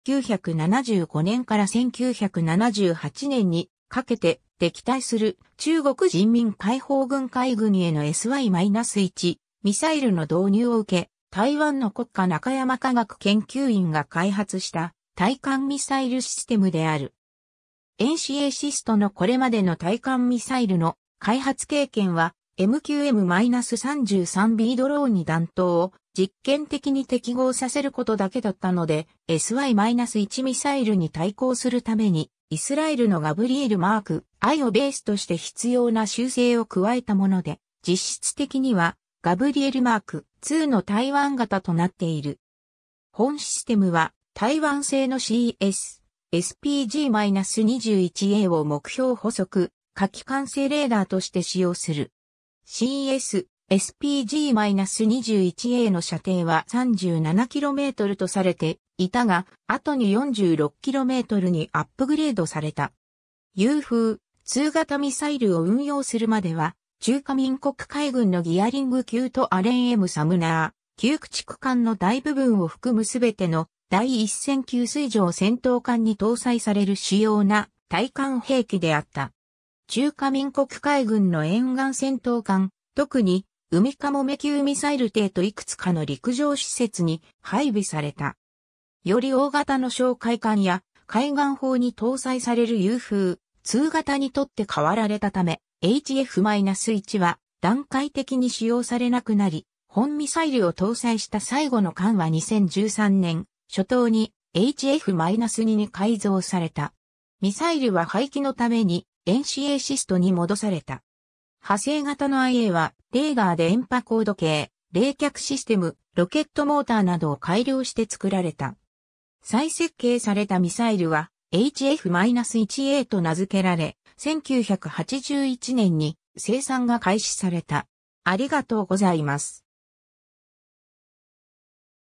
雄風I型 (ミサイル)
雄風I型（HF-1、Hsiung Feng I、中：雄風一型反艦飛彈）は、1975年から1978年にかけて、敵対する中国人民解放軍海軍へのSY-1ミサイルの導入を受け、台湾の国家中山科学研究院（NCSIST）が開発した対艦ミサイルシステムである。
 NCSISTのこれまでの対艦ミサイルの開発経験は、MQM-33Bドローンに弾頭を実験的に適合させることだけだったので、SY-1ミサイルに対抗するためにイスラエルのガブリエルMk.Iをベースとして必要な修正を加えたもので、実質的にはガブリエルMk.IIの台湾型となっている（中華民国海軍もアレン・M・サムナー級駆逐艦3隻を修復するためにガブリエルMk.IIシステムを数セット輸入していたため、HF-Iをイスラエル製の兵器と互換性を持たせることが取り決められていた）。
 本システムは、台湾製の CS/SPG-21A を目標捕捉・火器管制レーダーとして使用する。
CS/SPG-21Aの射程は37kmとされていたが、後に46kmにアップグレードされた。
 雄風II型ミサイルを運用するまでは、中華民国海軍のギアリング級とアレン・M・サムナー級駆逐艦の大部分を含むすべての第一線級水上戦闘艦に搭載される主要な対艦兵器であった。
中華民国海軍の沿岸戦闘艦、特に海鴎級ミサイル艇といくつかの陸上施設に配備された。
より大型の哨戒艦や海岸砲に搭載される雄風II型に取って代わられたため、HF-1は（海鴎級ミサイル艇と一緒ともに）段階的に使用されなくなり、本ミサイルを搭載した最後の艦は2013年初頭にHF-2に改造された。
ミサイルは廃棄のためにNCSISTに戻された。
 派生型のIAは、レーダー電波高度計、冷却システム、ロケットモーターなどを改良して作られた。
再設計されたミサイルはHF-1Aと名付けられ、1981年に生産が開始された。